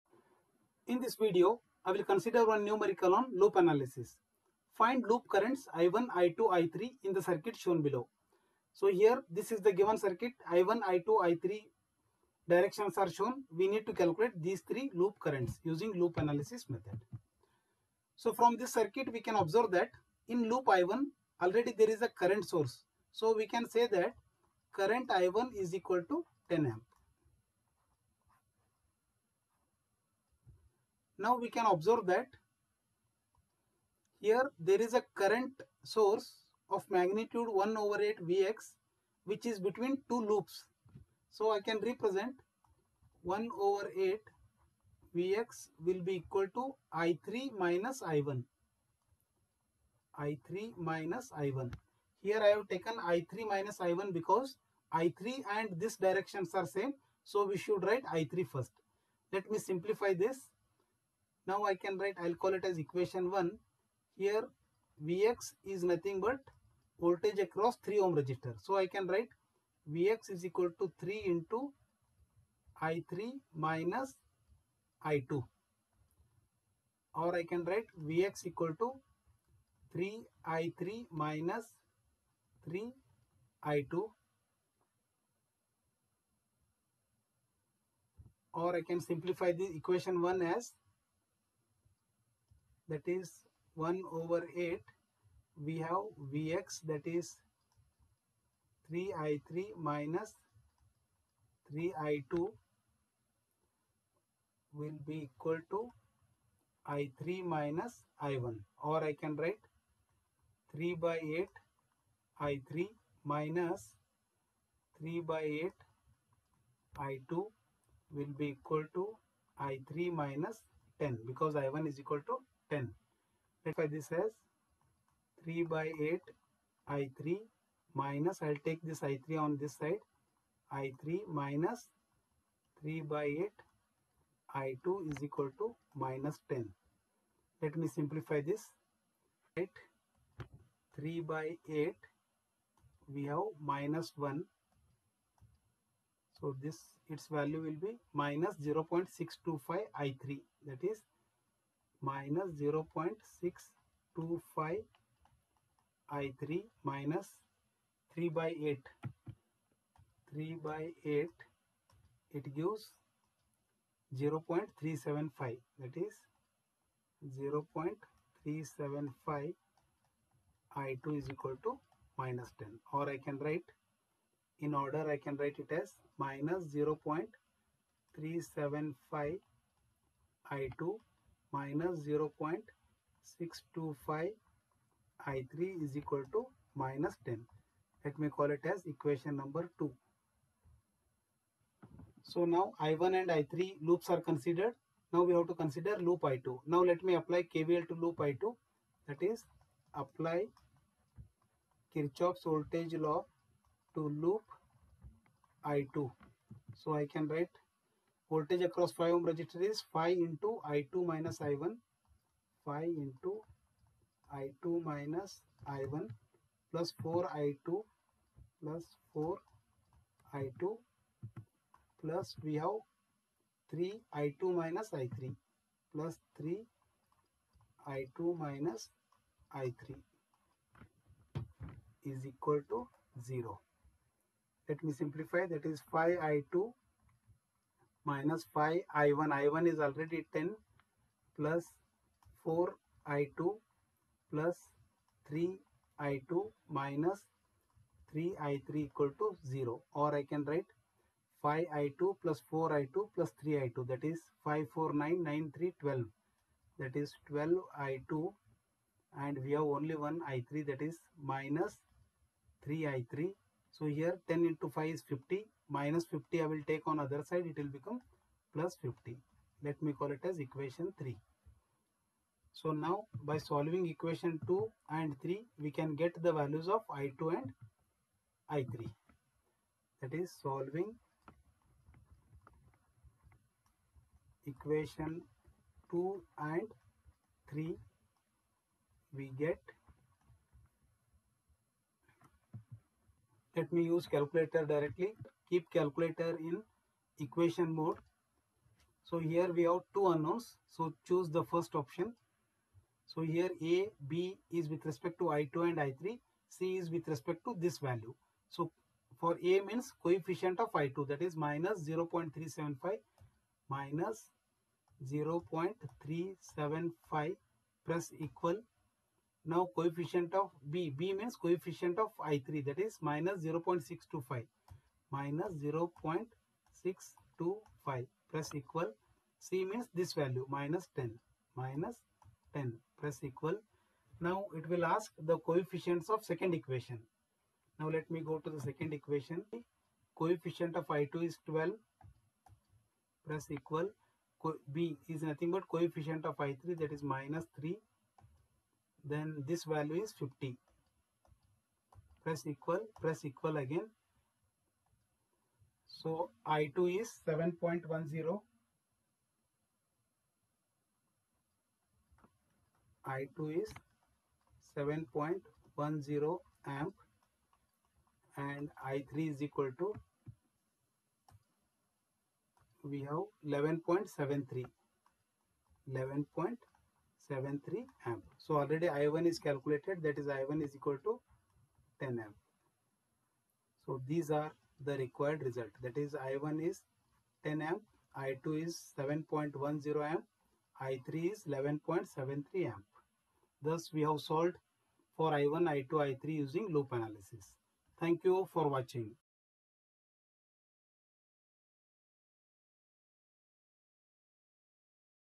Find loop currents I1, I2, I3 in the circuit.
BEC 304 Network analysis  Jan 2025 QP SOLUTION VTU